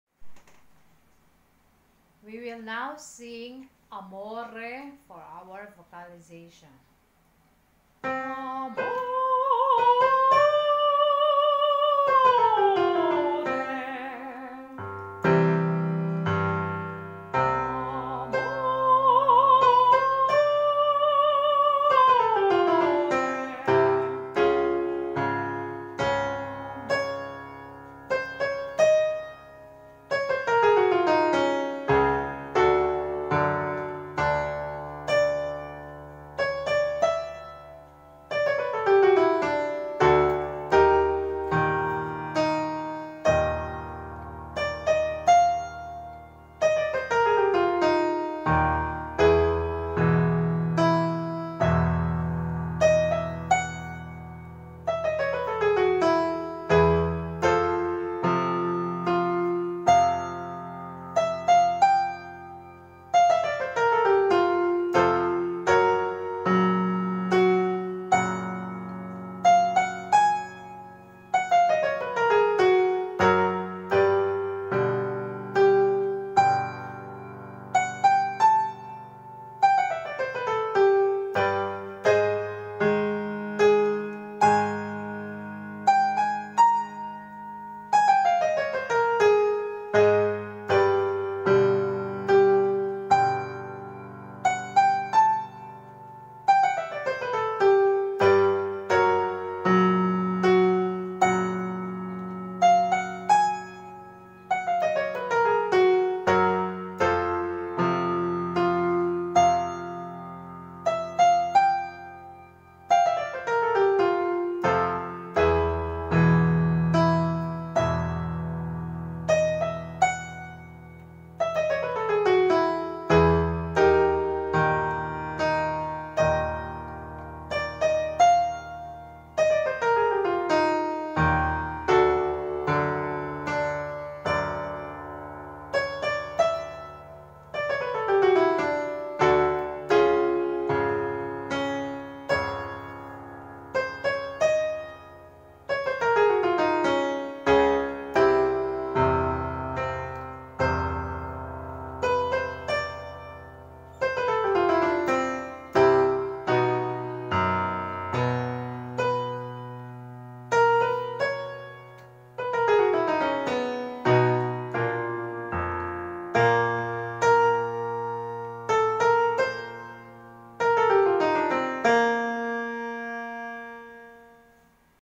Vocalization: Sing 'amore'
I am a voice professor from the Philippines.
This video is intended for my students but anybody can sing ng try this.
Enjoy singing!